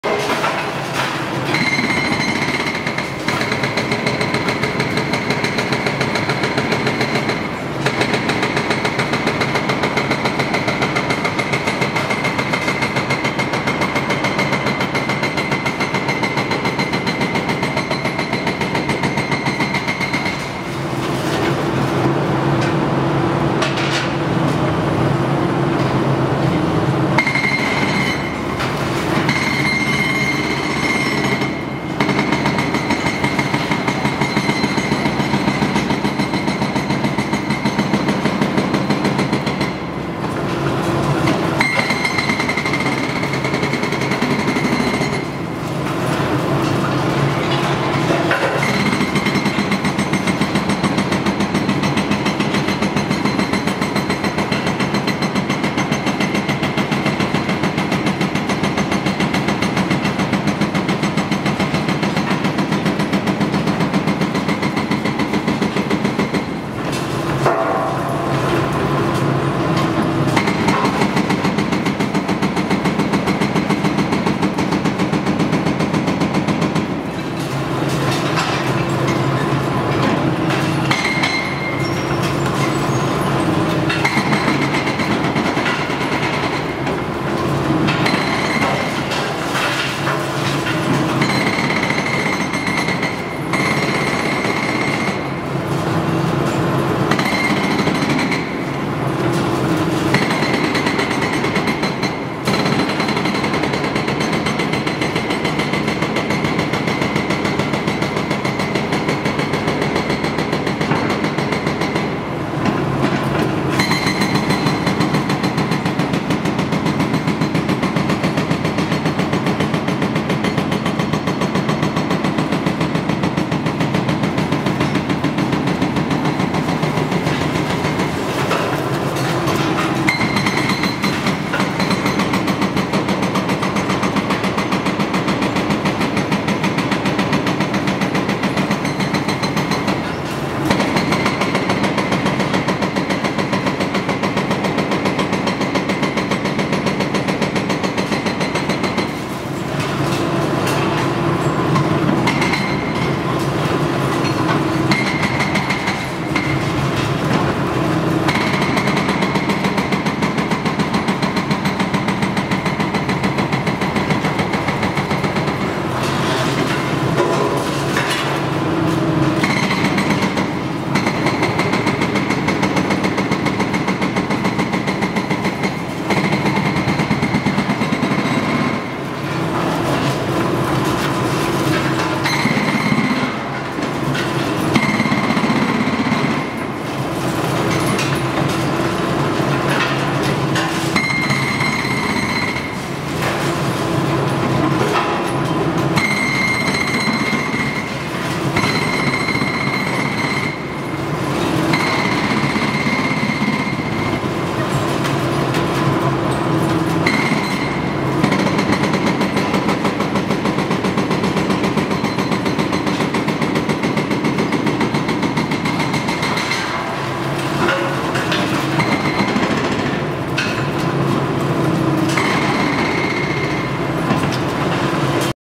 Kobelco SK350 NLC-9 with Montabert V45
Working at a demolition site in germany.